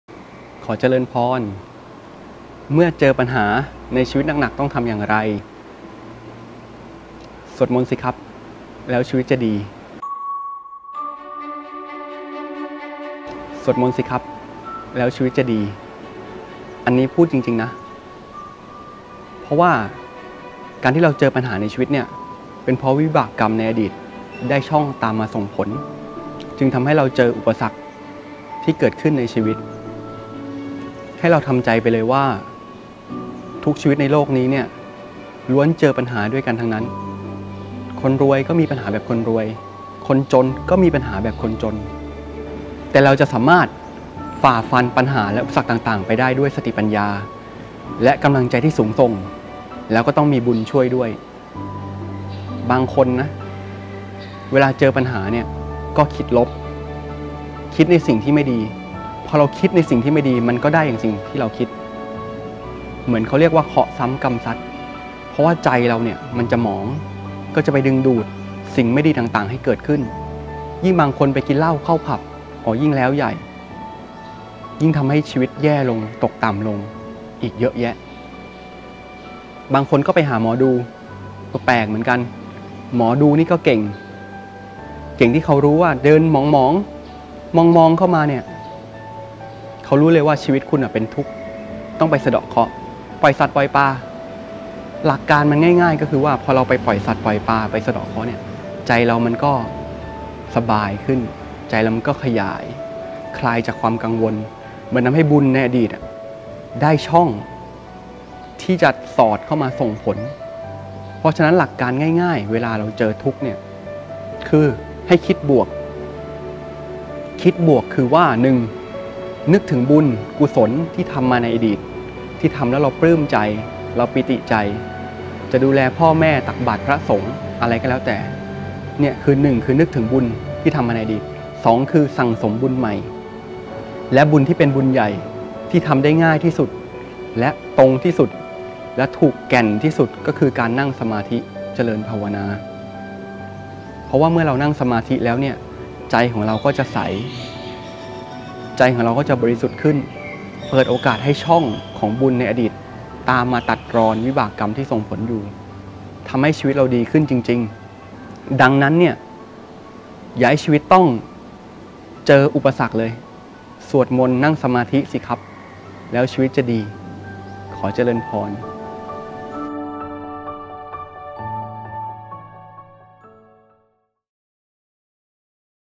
เมื่อเจอปัญหาหนักๆ ในชีวิตจะต้องทำอย่างไร ?
เมื่อฟังหลวงพี่เบนซ์จบ เราจบพบคำตอบทันที

แชร์รัวๆ